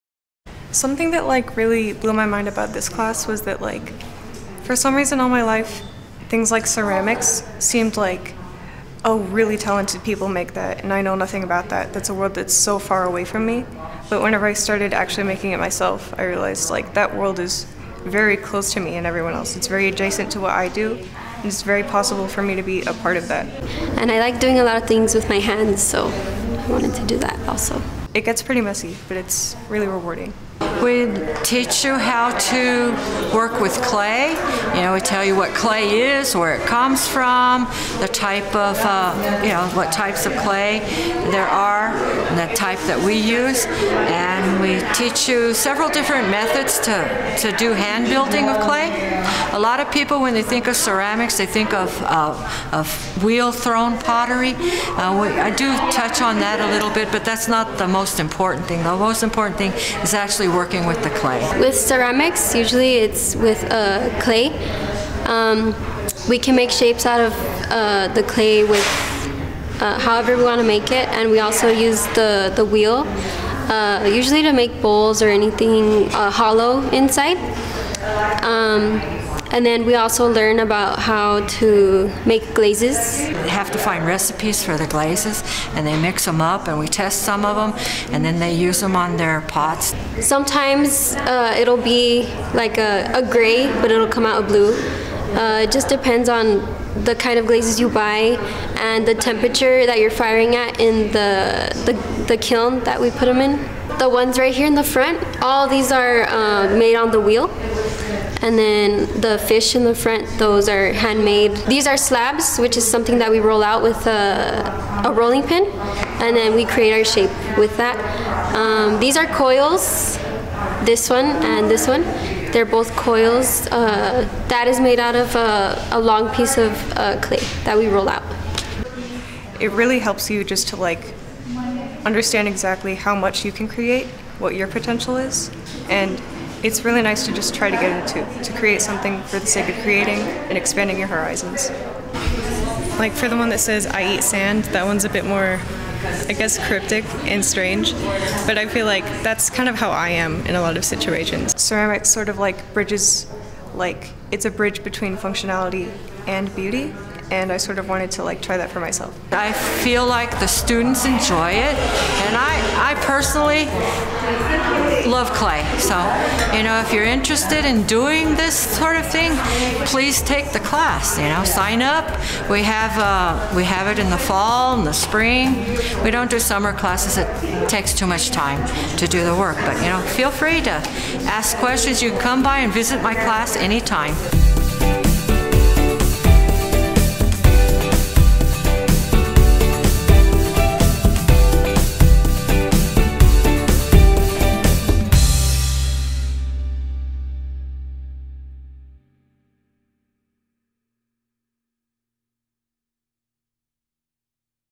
Inside EPCC Ceramics @ Valle Verde
Instructor Michelle Murphy and students talk about the Ceramic classes at the Valle Verde Campus.